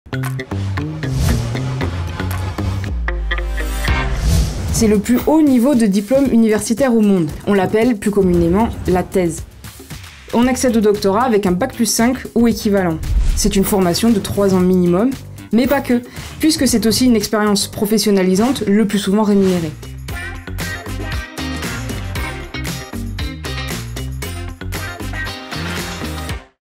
Doctorat, docto... WHAT ? ÉPISODE 1 "Le Doctorat, qu'est-ce que c'est ?"
Doctorat, docto... WHAT ? ÉPISODE 1
"Le Doctorat, qu'est-ce que c'est ?" 🤔📚
🔍 Vous voulez en savoir plus sur le plus haut niveau de diplôme universitaire au monde ? Ne cherchez plus, cet épisode est fait pour vous !

 / posts
X (Ex Twitter) :

 / univ_bfc

🙏 Merci à toutes les personnes et structures ayant contribué à la réalisation de cette vidéo, notamment :
Doctorants / Docteure :
Johan ACHARD · Jehanne SECK · Tiffany AMAT · Tom CATEL · Yağmur OZTURK
Fatima BENKIRANE ·Maxime FEDERICO · Clémence LAVIGNE
Encadrants de thèse :
Hans JAUSLIN · Nathan CROMBEZ
Établissements d'enseignement supérieur :
Université de Bourgogne
Université de Franche-Comté
Université de Technologie de Belfort-Montbéliard
L’Institut Agro Dijon
SUPMICROTECH - École Nationale Supérieure de Mécanique et des Microtechniques
Laboratoires de recherche :
Centre de Recherches Interdisciplinaires et Transculturelles - CRIT
Connaissance et Intelligence Artificielle Distribuées - CIAD-LAB
Franche-Comté Électronique Mécanique Thermique et Optique - FEMTO-ST
Formation et Apprentissages Professionnels - FoAP
Laboratoire de recherche Intégratives en Neurosciences et psychologie Cognitive - LINC
Laboratoire Interdisciplinaire Carnot de Bourgogne - ICB
Maison des Sciences de l'Homme et de l'Environnement Claude-Nicolas Ledoux - MSHE
Procédés Alimentaires et Microbiologiques - PAM

🎥 Réalisation Kaméléon Prod - Novembre 2023